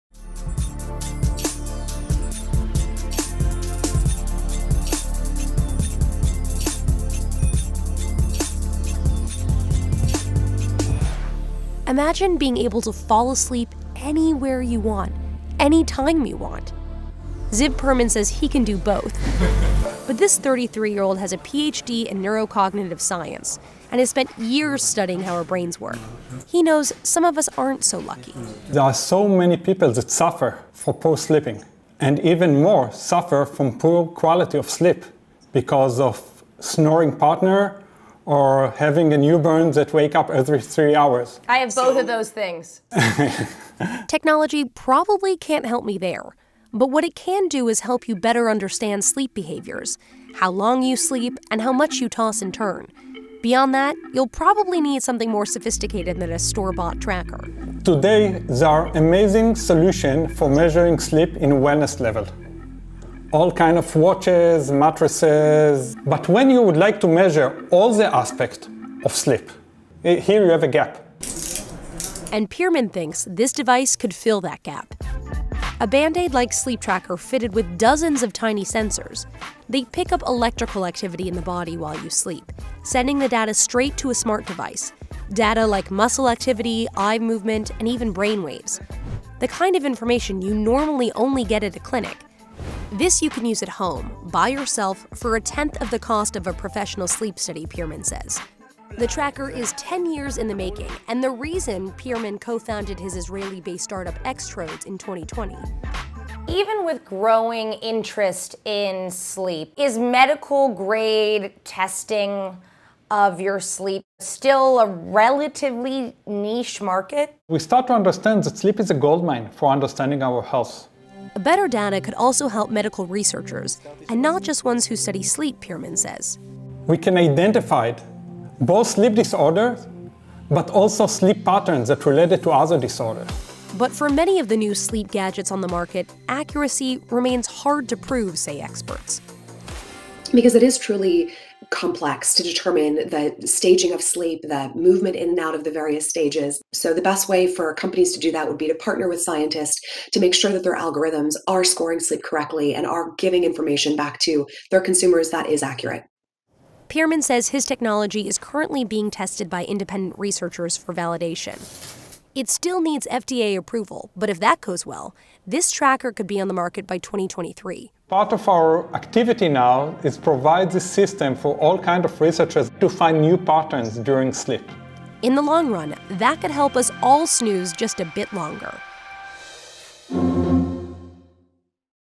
النوم "منجم ذهب لفهم صحتنا".. كيف تستخدم التكنولوجيا للنوم بشكل أفضل؟
تخيّل أنك قادر على النوم في أي مكان تريد وفي أي وقت تريد. يقول زيف بريمن إنه يستطيع فعل الأمرين معًا. ويعتقد أن الجهاز الذي ابتكرته شركته X-trodes يمكن أن يملأ فجوة في السوق لقياس جميع جوانب النوم. وهو جهاز تعقب يشبه الضمادات اللاصقة مزوّد بعشرات من أجهزة_الاستشعار الصغيرة التي تلتقط النشاط_الكهربائي في الجسم أثناء النوم، وترسل البيانات مباشرة إلى جهاز_ذكي .